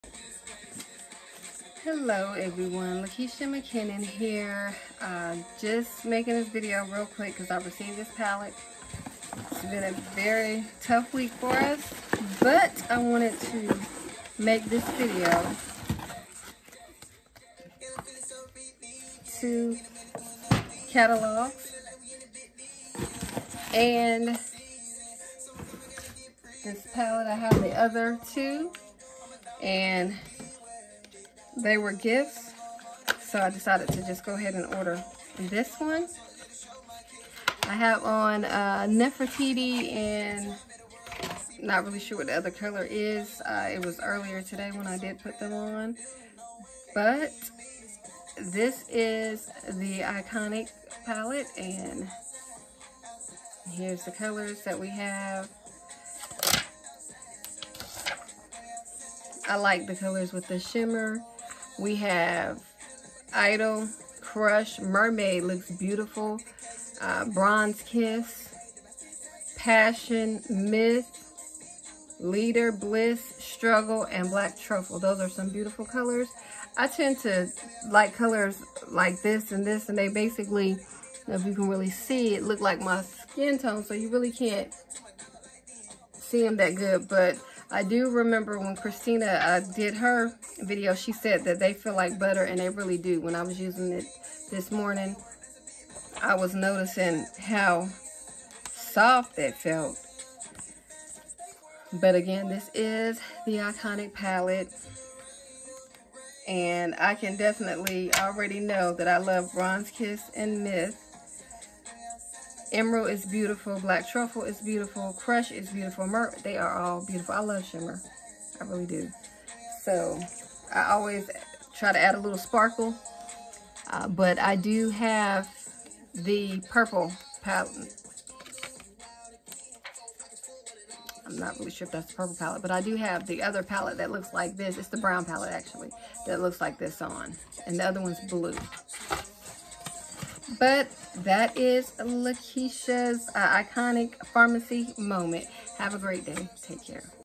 FARMASI ICONIC EYESHADOW PALETTE 12 SHADES
Iconic shades in a palette. 12 intensely-pigmented shades are designed for every occasion and every mood!

Mix & match to suit your every mood!  The colours and textures look incredible on every skin tone A dance floor of fun and divine shades including shimmer!  Rich in pigments and emollients for a velvety-smooth, soft finish. Easy to blend.